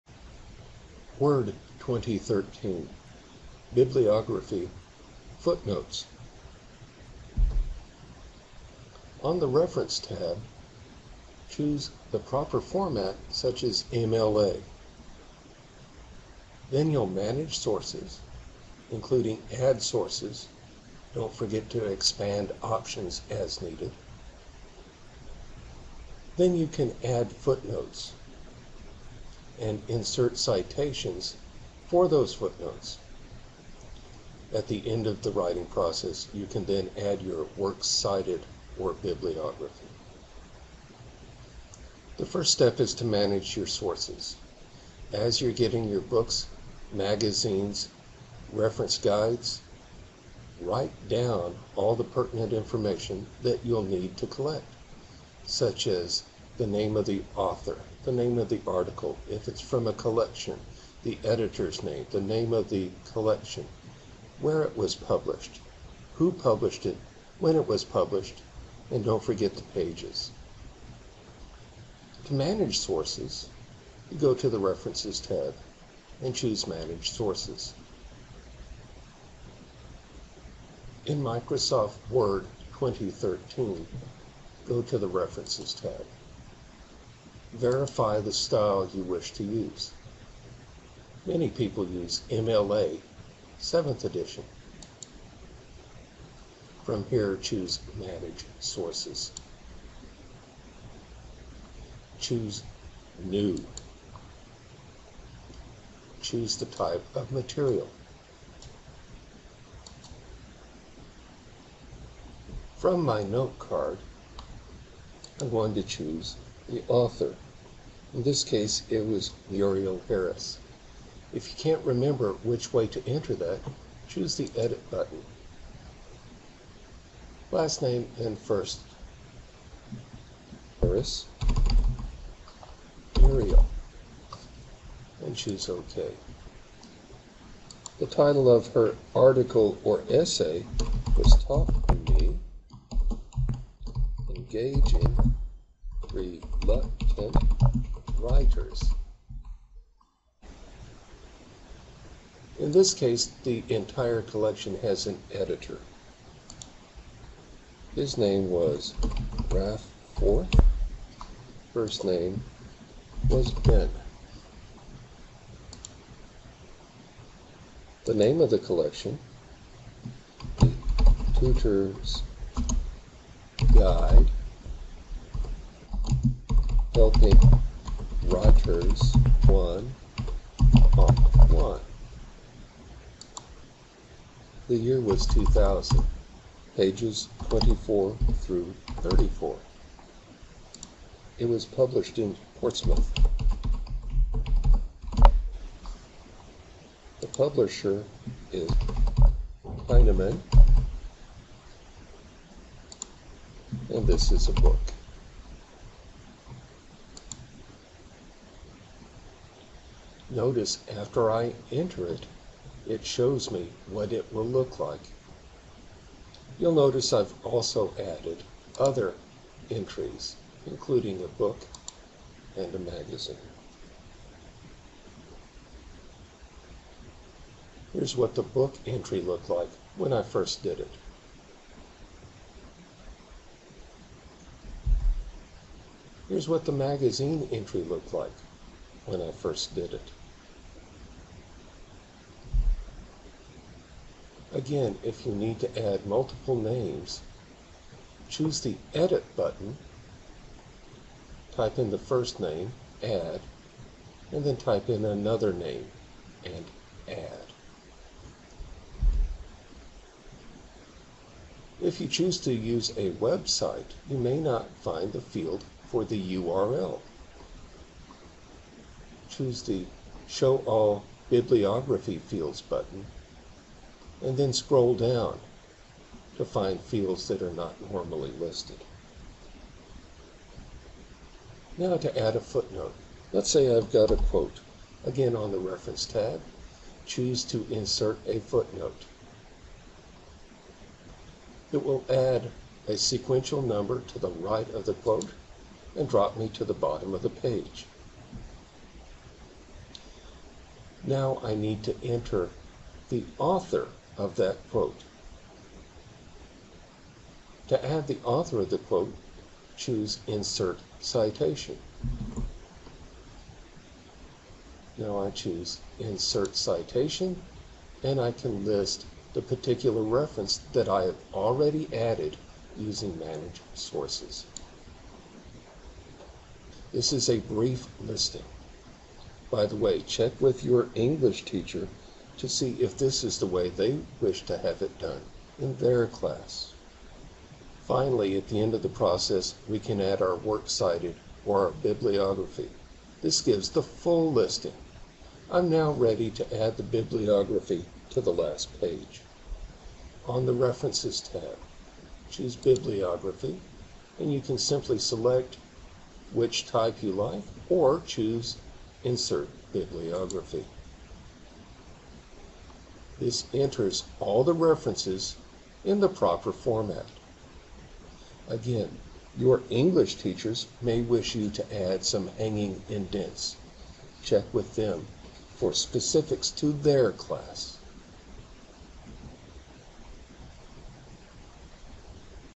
Word 2013 Bibliography-Footnotes
Using Word 2013 to add Footnotes, citations, and bibliographies to papers